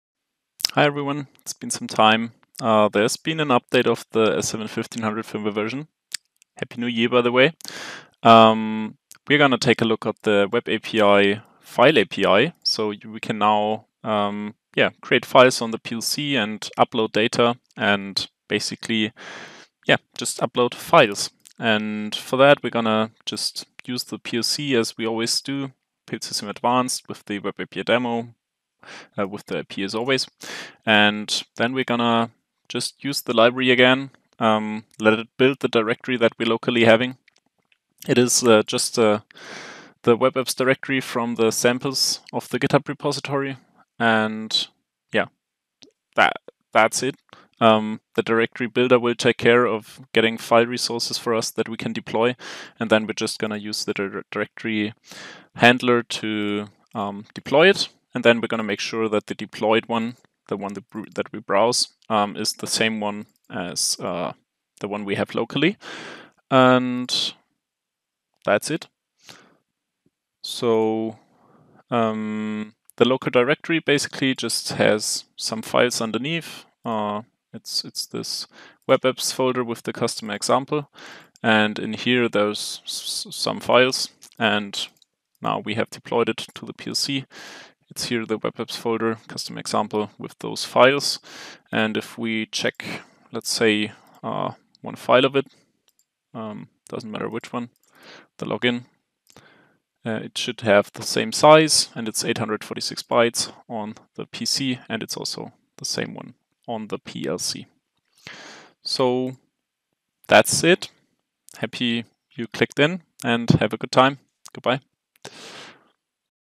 S7-1500 Web-API: Files & Directories
I wish you all a happy new year and a pleasant 2023!

My name is Max and in this video I will show you how you can deploy a local (windows) directory using the C# .NET Client Library for the Siemens Simatic S7-1500/S7-1200 Web-API. (Currently only supported by the S7-1500)

0:00 Intro - new Firmware
0:25 Configuration Overview
0:40 Start Deployment
1:15 Local Directory
1:27 Directory on PLC
1:40 Comparison File on PC = PLC

Enjoy, stay healthy and be nice to eachother!